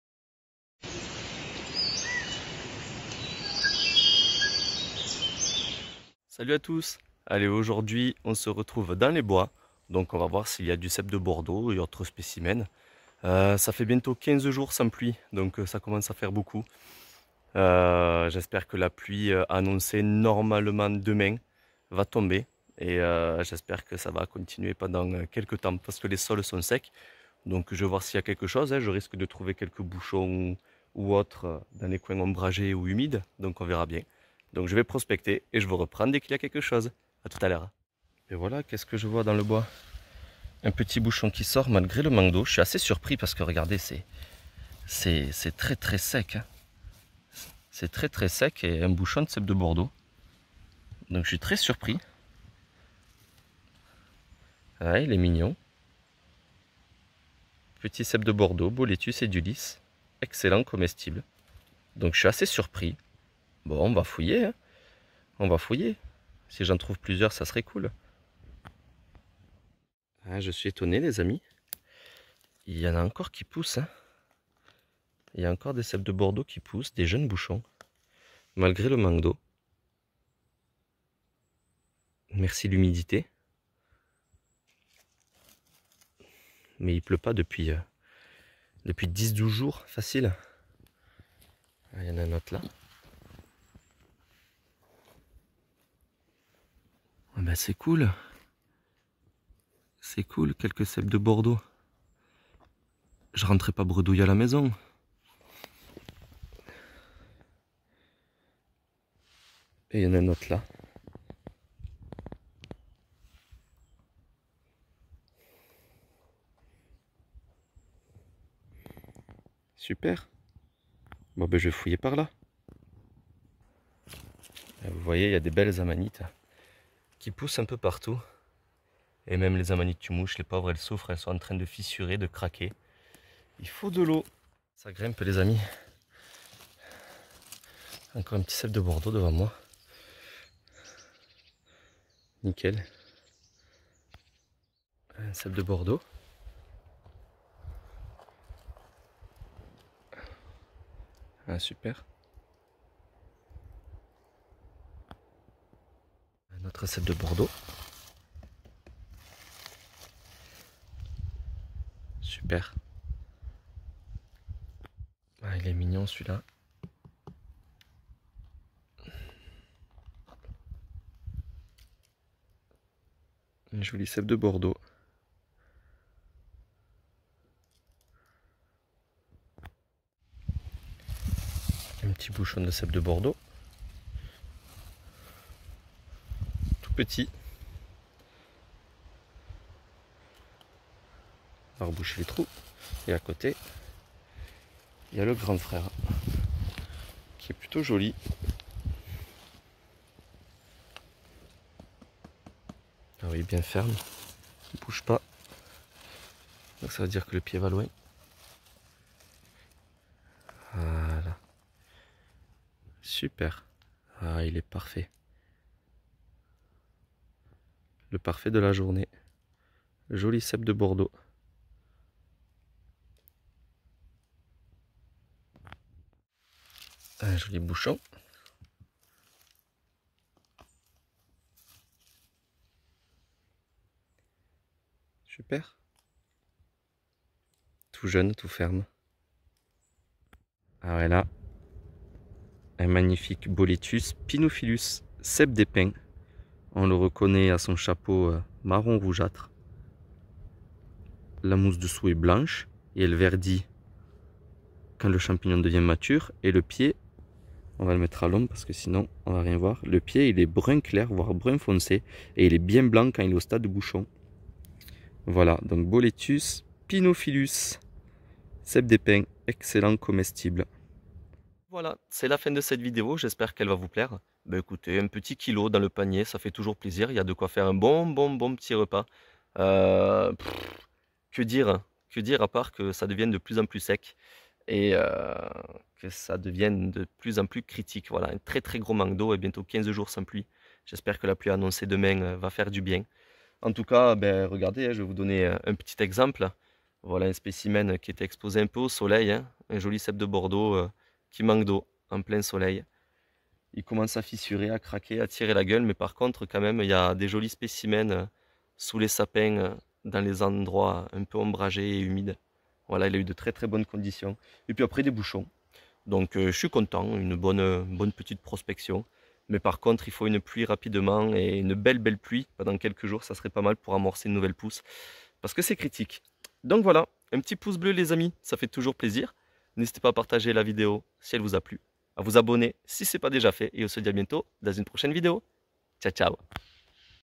Cèpes de Bordeaux 😍 : Petite ceuillette après 14 jours sans pluie !
Salut à tous ! gros manque d'eau chez nous ... j'espère que sa ira mieux dans les prochains jours pour des vidéos encore plus belles ! bon visionnage 😊✅